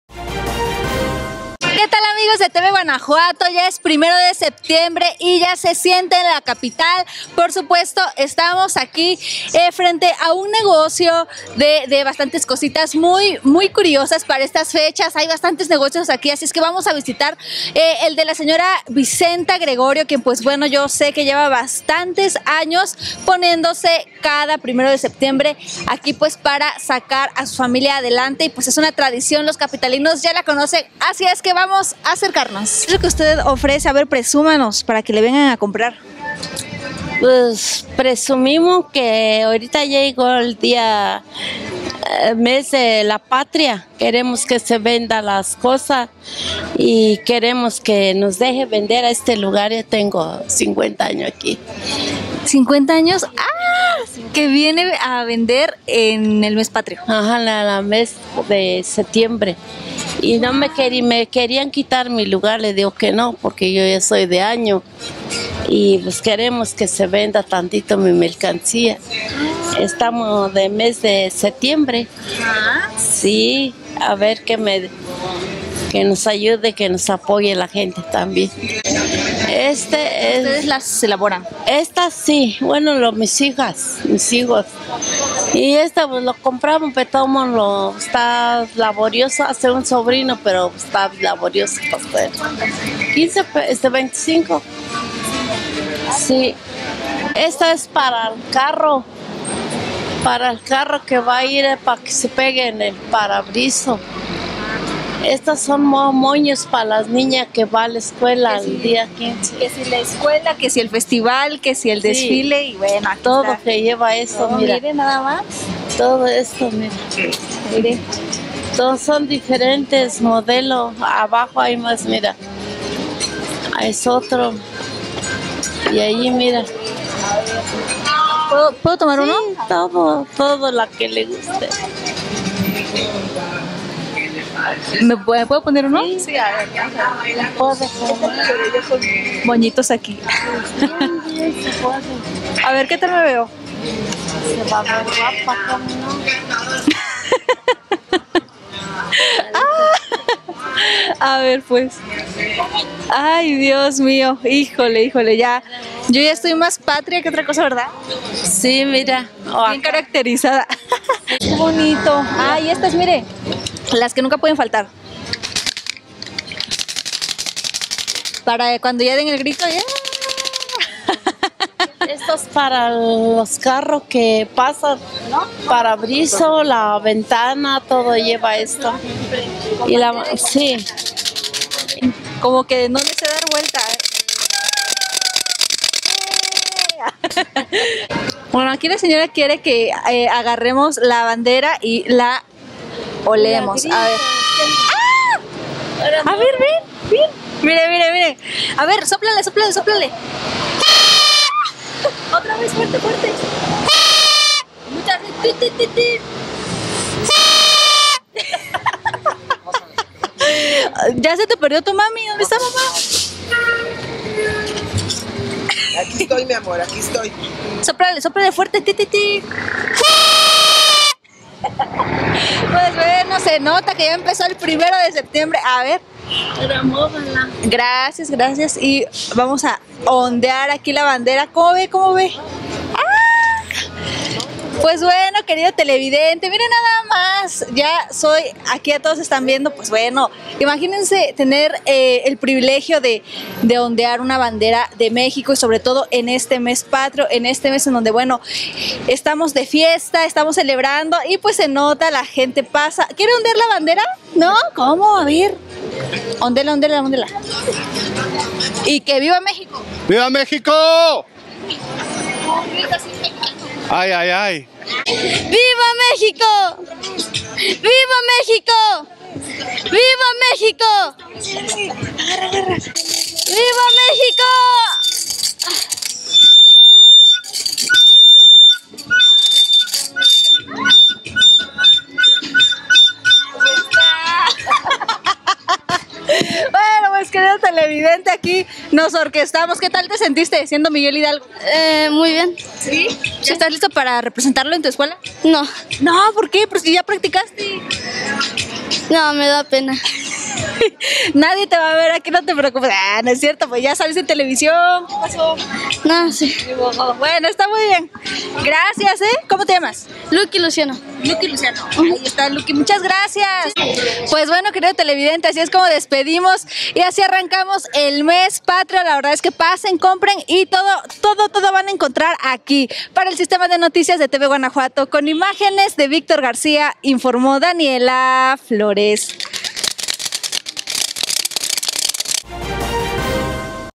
Sondeo ciudadano, puestos del 15 de septiembre 🇲🇽
Los tradicionales puestos del 15 de septiembre 🇲🇽 ya se hicieron presentes en nuestra ciudad para darle color a este mes patrio.